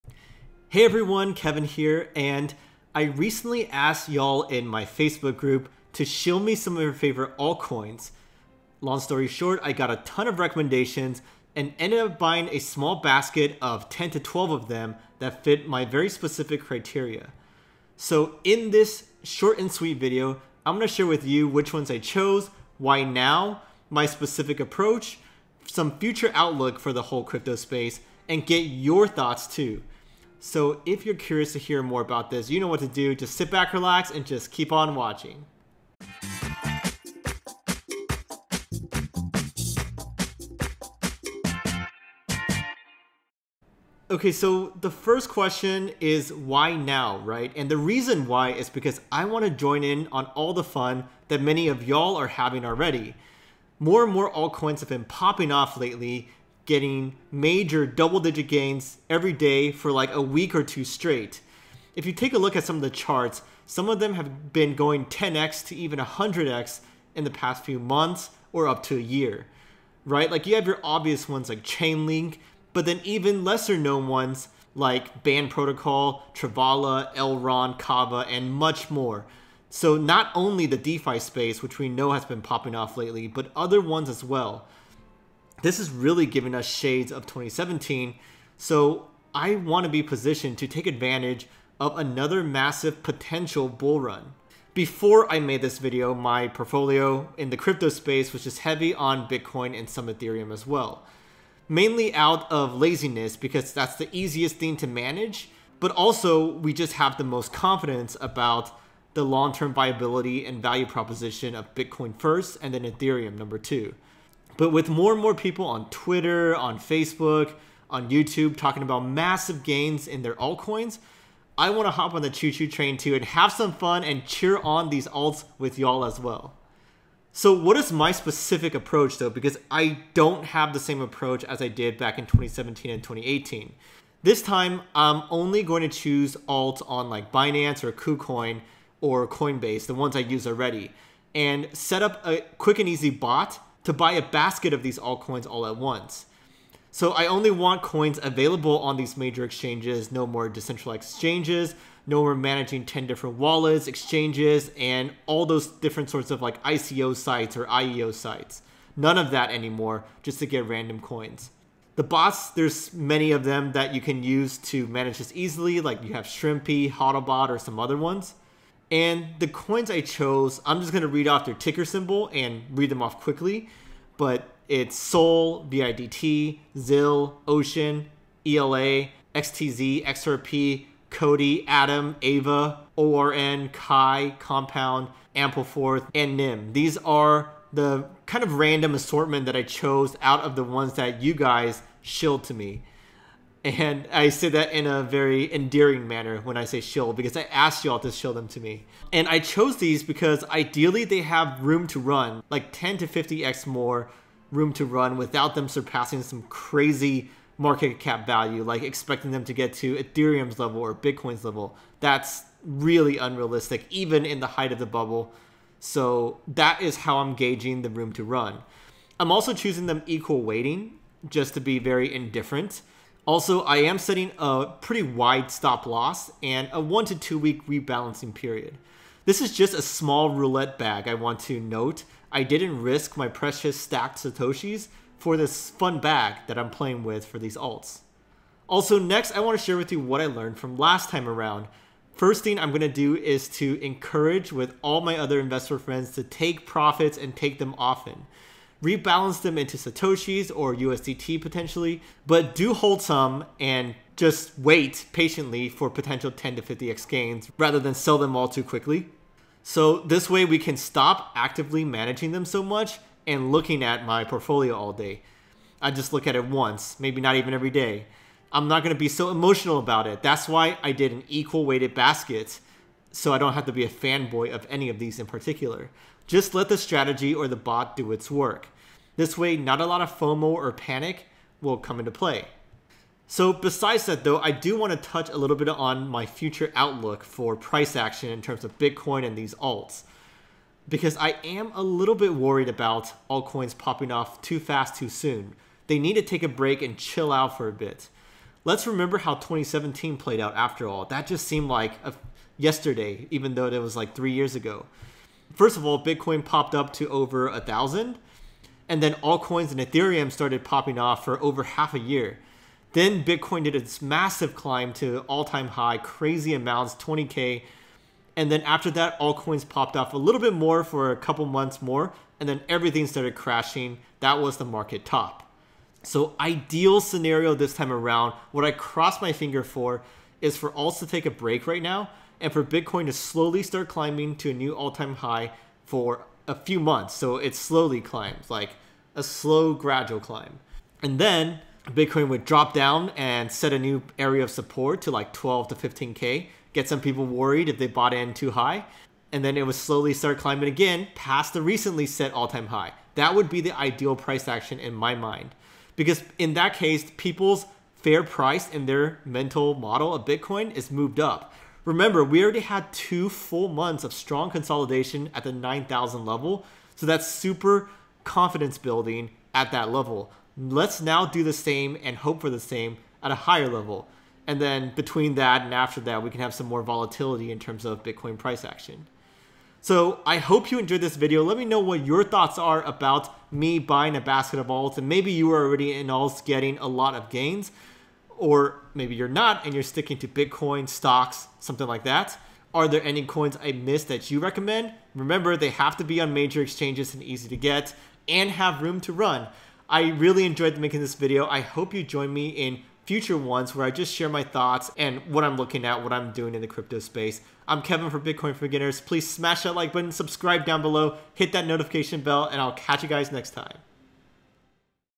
My 2020 Altcoin Investing Strategy!
Recently I pulled the trigger on purchasing a basket of some random altcoins/cryptocurrencies in equal weights. I finally had to get in on the fun that altcoin investors were experiencing in the past few weeks or months. We've seen some altcoins rocket 10-50x already and I was mostly in the slower moving Bitcoin and Ethereum. In this video I want to share my strategy, my approach, my criteria, the actual altcoins that I chose, what I learned from the last bull cycle, and get YOUR recommendations too! So if you are currently looking at the crypto markets deeply, let me know YOUR thoughts after watching my video!